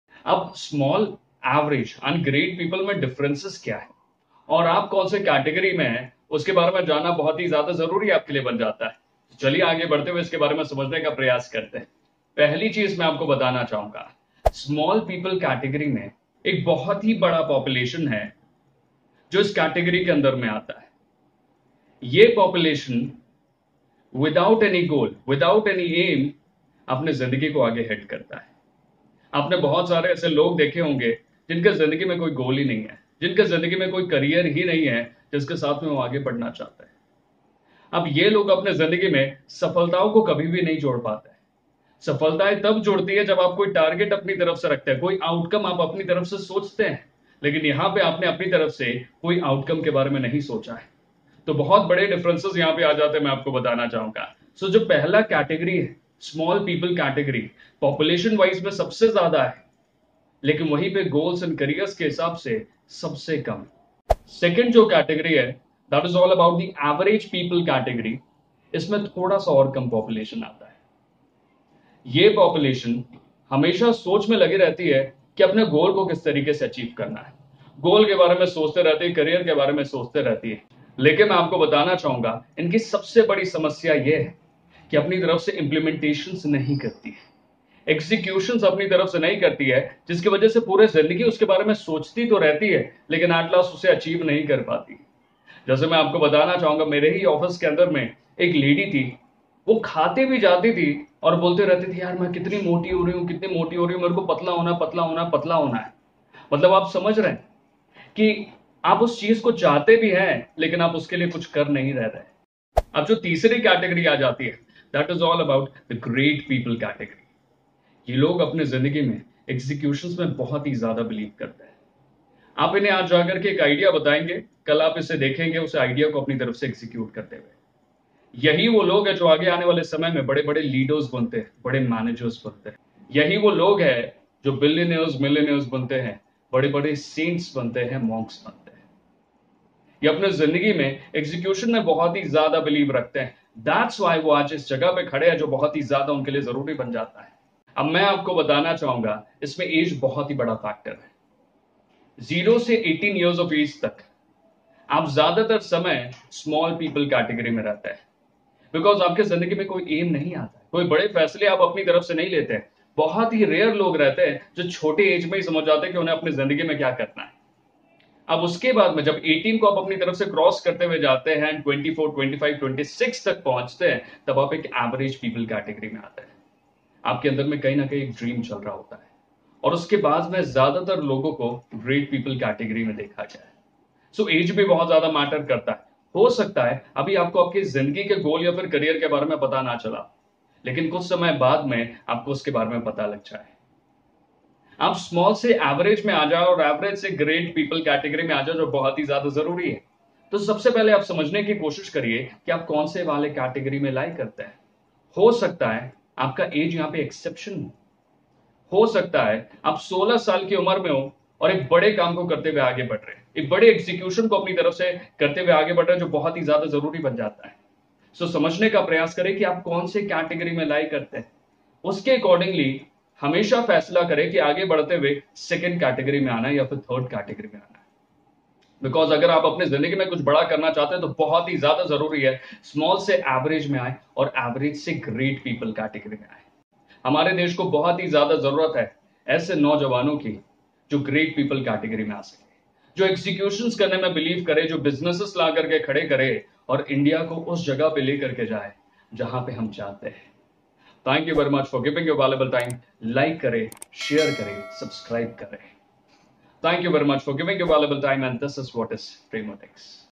आप के अंदर किस तरह का इंसान छुपा हुआ हे ? || Premodex
Here Mr. Prem Piyush (Motivational Speaker, Sales Coach, Life Coach, Business coach and Founder Of Premodex) Talks about :

3 Kinds Of People :

1.Small People
2. Average People
3. Great People

*A detailed video on the above points*

Kindly watch this video till the end and share your valuable feedback through comment.